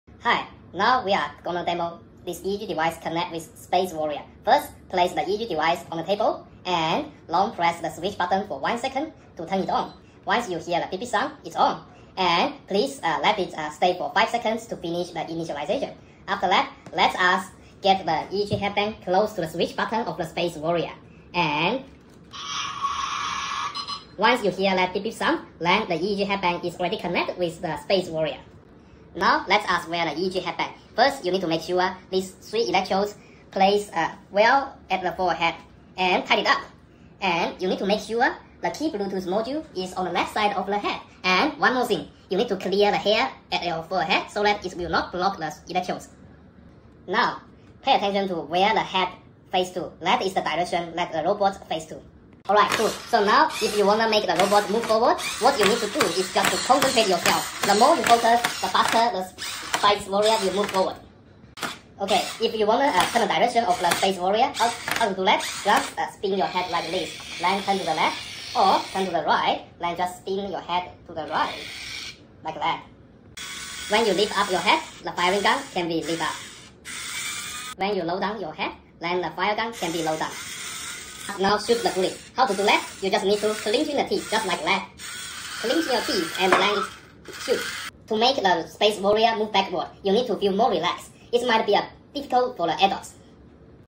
Space Warrior----Spider Robot RAP
Check this out,let spider robot dance in rap.